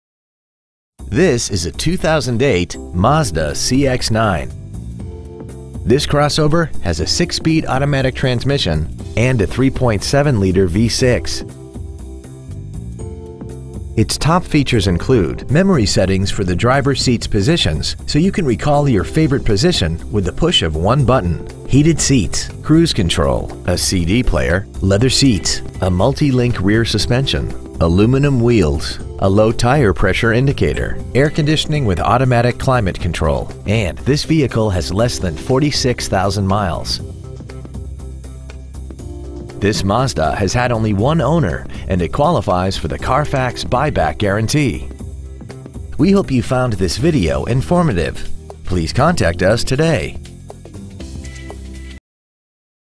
Pre-Owned 2008 Mazda CX-9 West Palm Beach FL
Year : 2008

 Make : Mazda

 Model : CX-9

 Engine : Gas V6 3.7L/227.4

 Trans . : Automatic

 Exterior : Sparkling Black Mica

 Miles : 45,007

 Interior : Sand

 Stock : 30392

 Offleaseonly.com

 3531 Lake Worth Rd

 Pre-Owned 2008 Mazda CX-9 Lake Worth FL